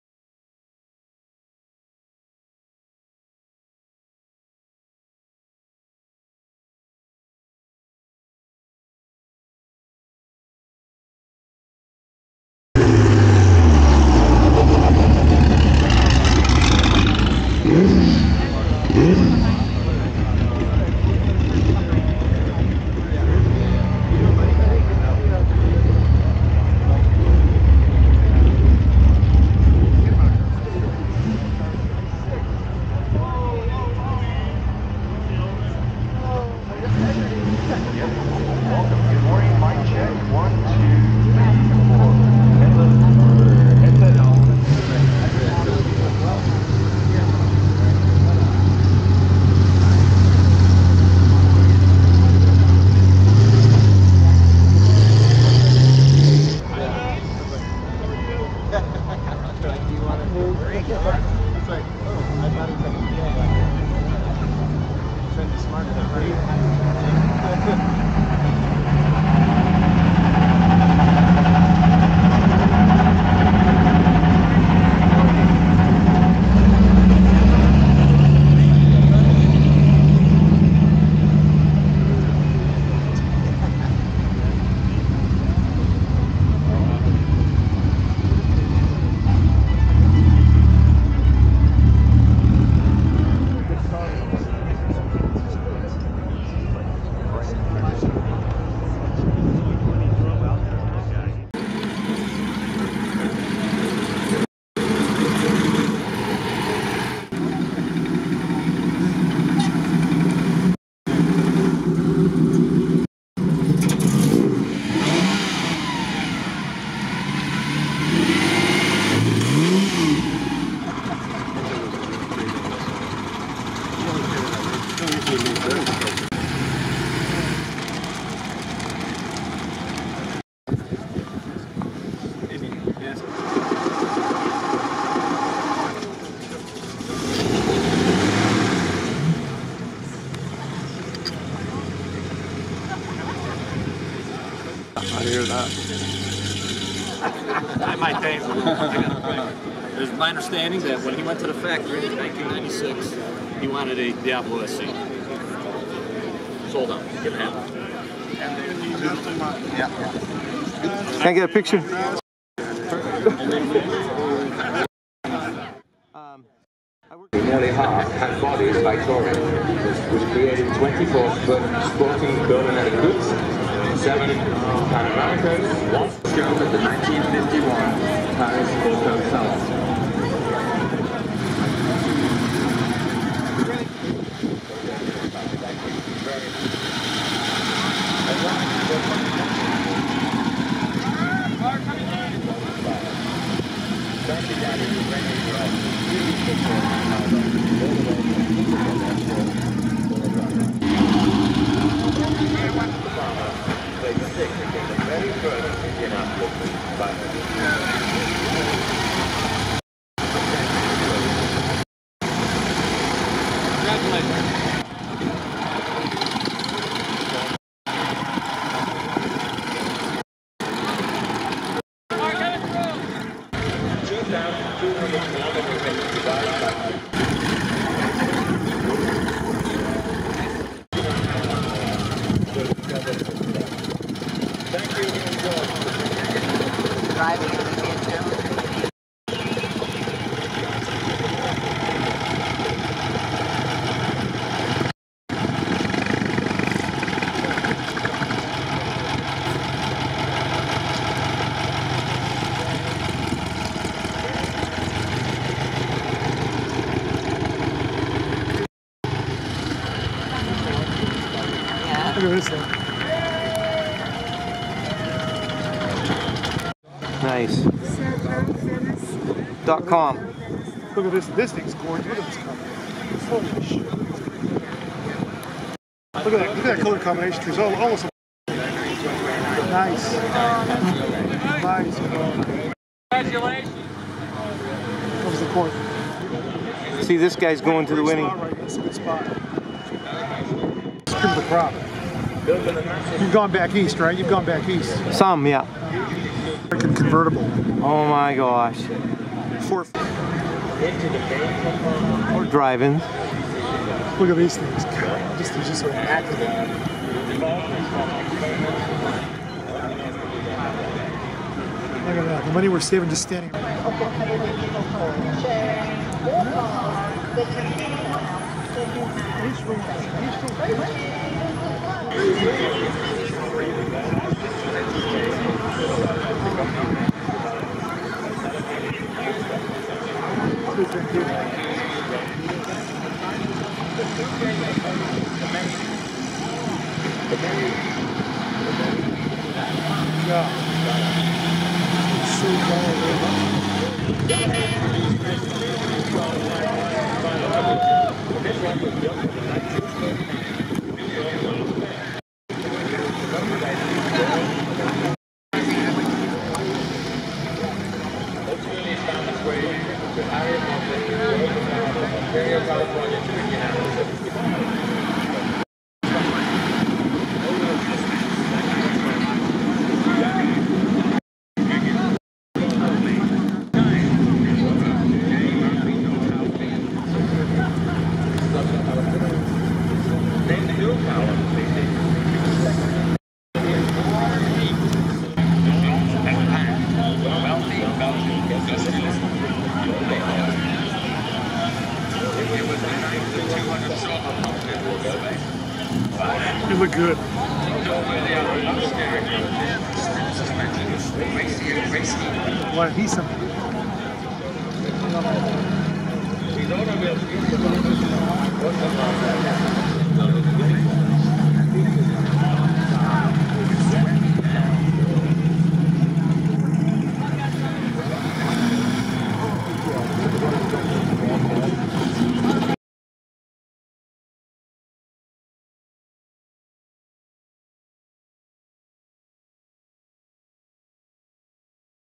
2023 Pebble Beach Concours d Elegance Part 001
The cars first arriving and some shots throughout the day.